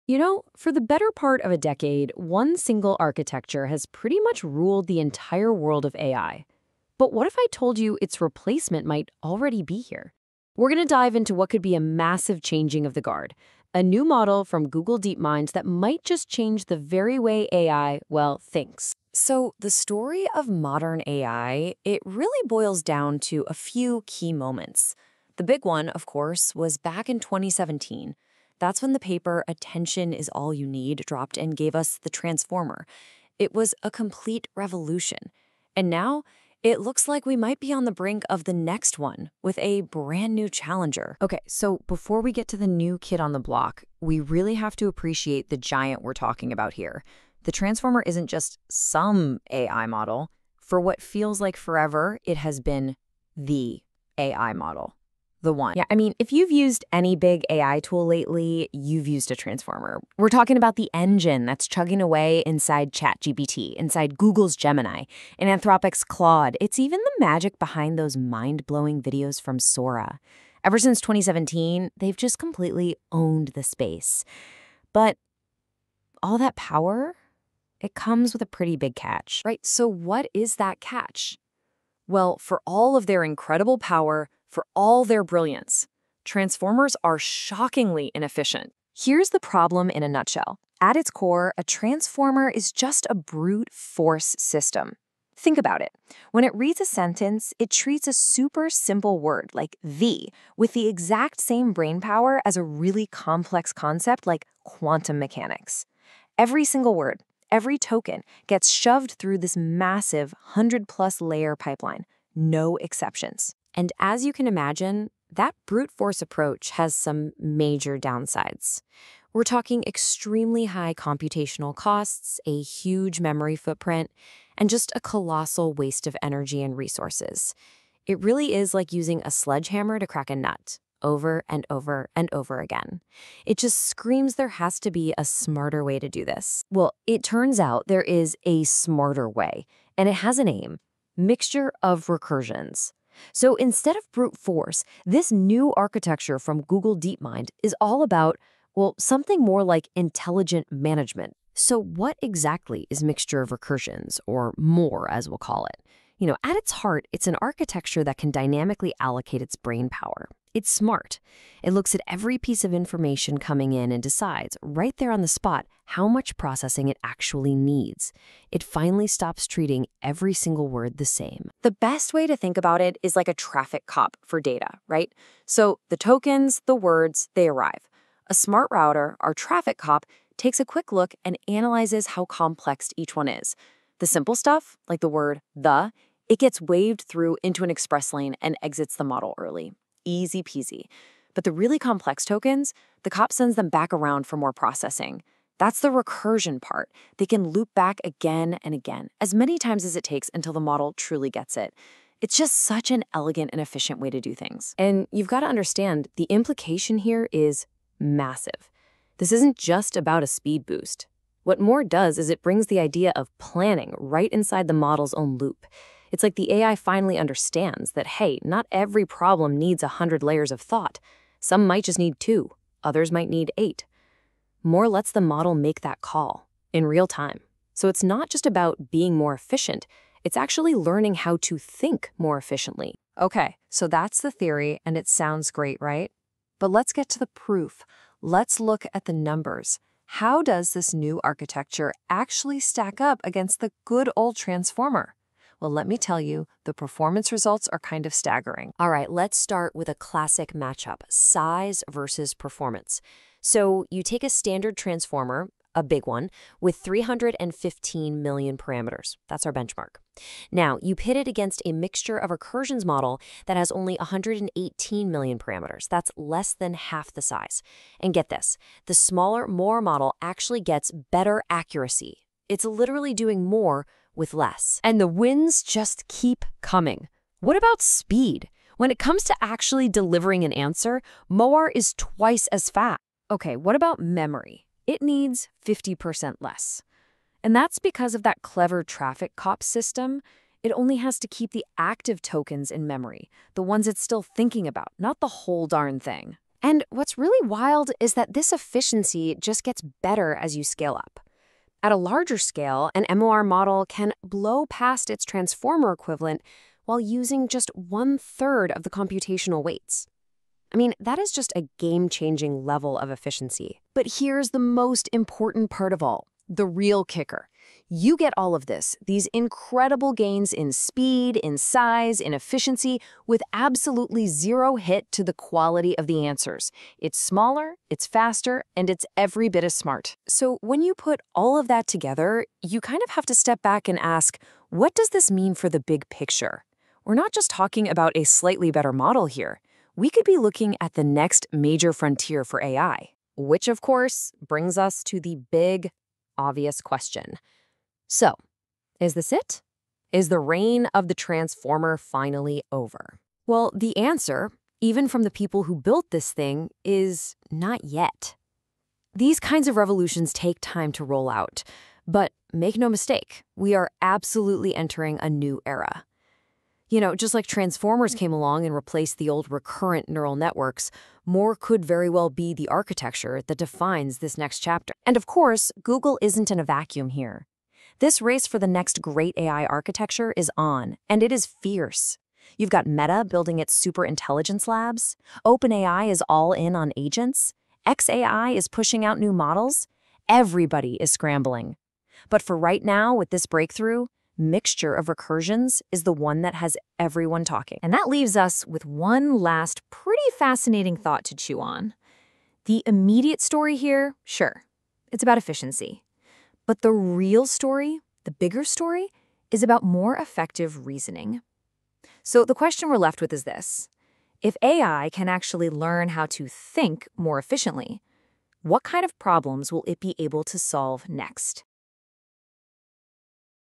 MoR: Google DeepMind's New Architecture | Faster, Smarter, and the Future of AI Beyond Transformers
In 2017, Google Brain gave the world Transformers, an architecture that made groundbreaking AI models like Chat GPT, Claude, Gemini, and even Sora possible. Now, in 2025, Google DeepMind introduces a revolutionary new architecture that might just be their successor: Mixture of Recursions, or MoR.

Discover why MoR is being hailed as leaner, faster, and possibly the future of AI. This isn't magic; it's brilliant design that rethinks how LLMs reason. Instead of processing every word through the same 100+ layers, MoR's smart router acts like a traffic cop, letting simple words exit early while complex ones go for a second or third loop, ensuring computation is only used where needed.
The benefits are astounding:
• Two times faster inference.
• 50% smaller memory needs.
• Half the training compute.
• Better performance than much bigger vanilla models.

MoR doesn't hold memory like Transformers; only active tokens get cached, leading to massive RAM savings. In rigorous tests, an 118 million MoR model outperforms a 315 million Transformer on few-shot accuracy, and at 1.7 billion scale, MoR excels with just one-third the weights. It's not just more efficient; it's smarter, allowing the model to decide in real-time how much computation each word actually needs.

Is this the end of Transformers? Not yet, but we are entering a new era. Just as Transformers replaced recurrent neural networks, MoR could become the next frontier for AI. Google seems to be writing the book on modern AI once again with this fast, memory-smart, recursively thinking architecture that might lead us into the next era of intelligence.